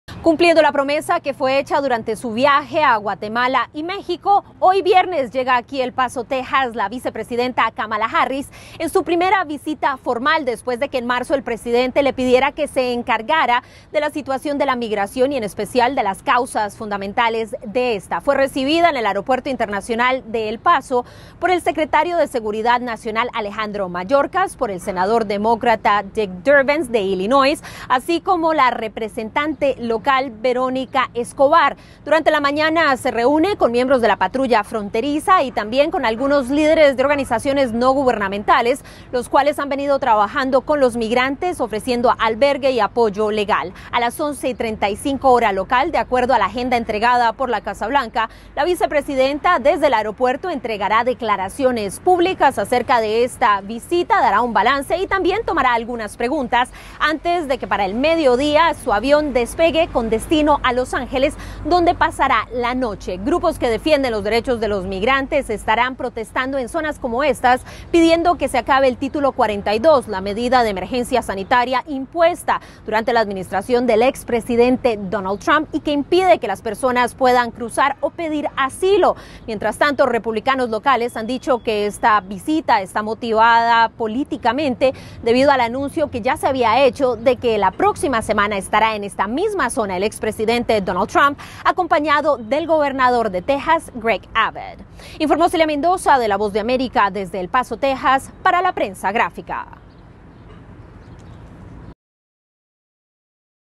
Kamala Harris visita la frontera entre México y Estados Unidos
Harris, se reunirá con inmigrantes detenidos por la Patrulla Fronteriza

¡Visítenos!